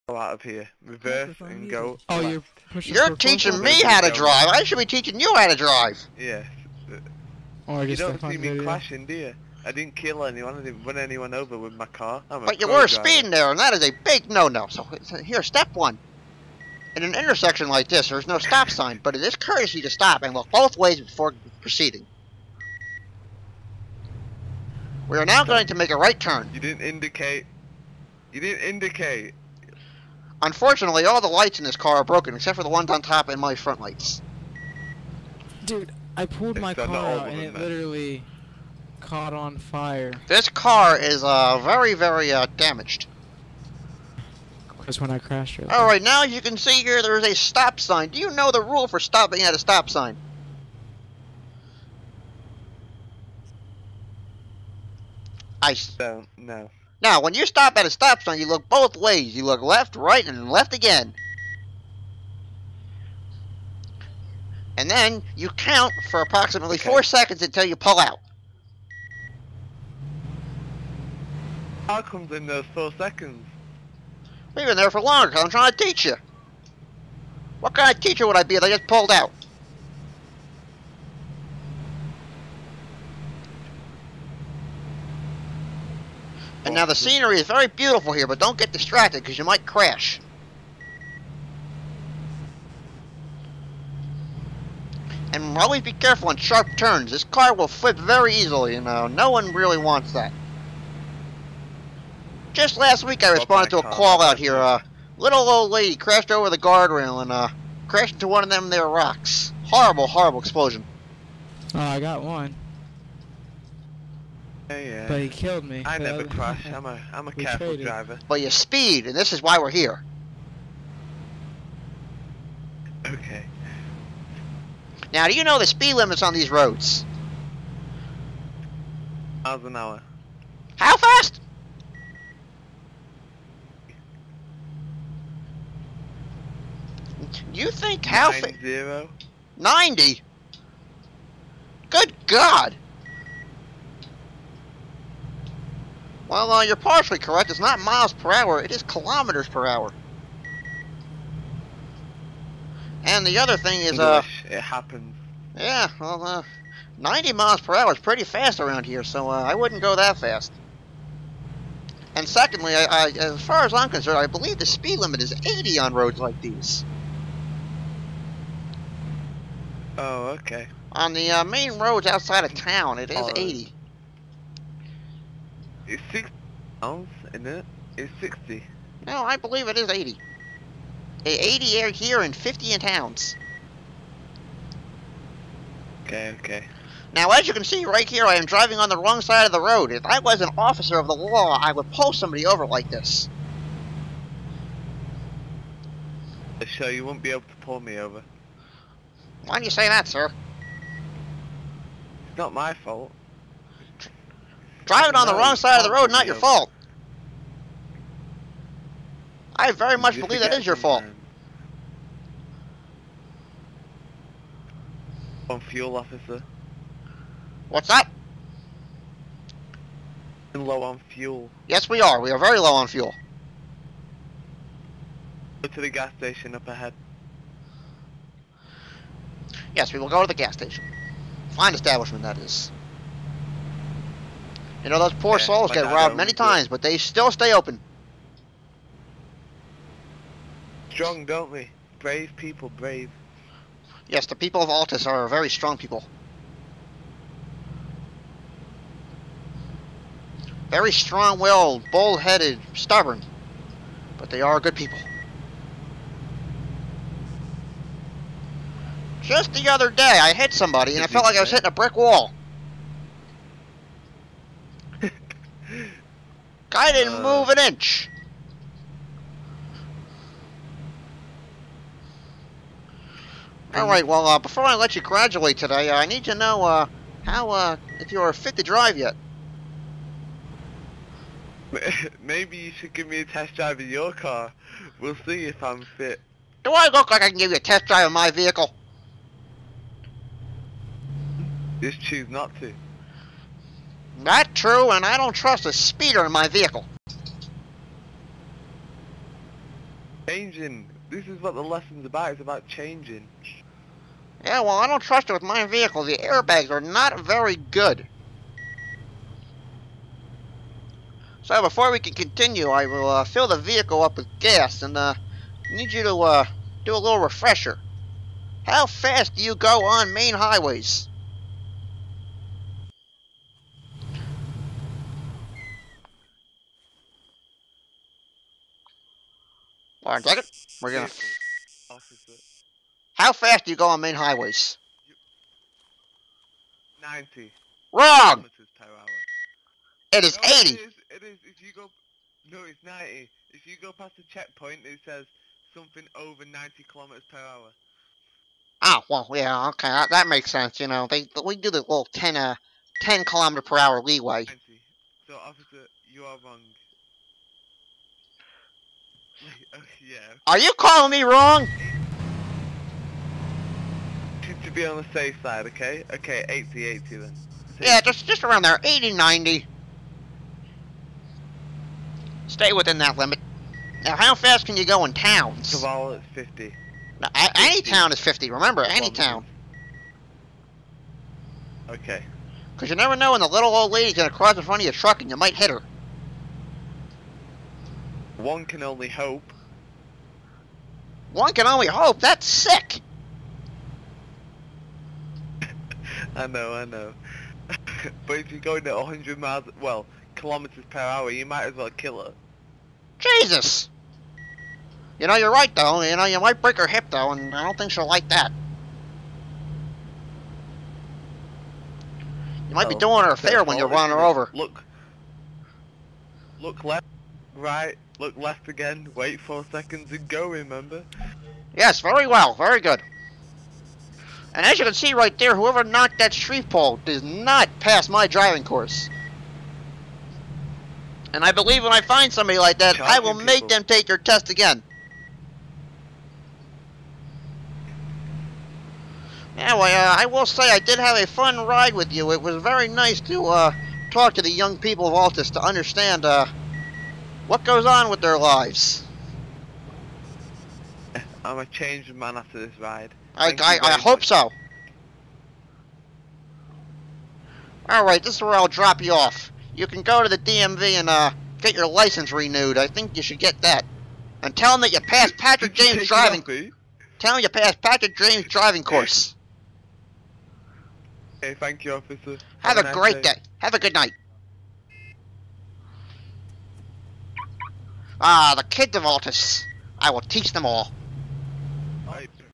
Patrick James' Driving School
This is the reason why you don't want me teaching you how to drive... Cause I SUCK AT IT!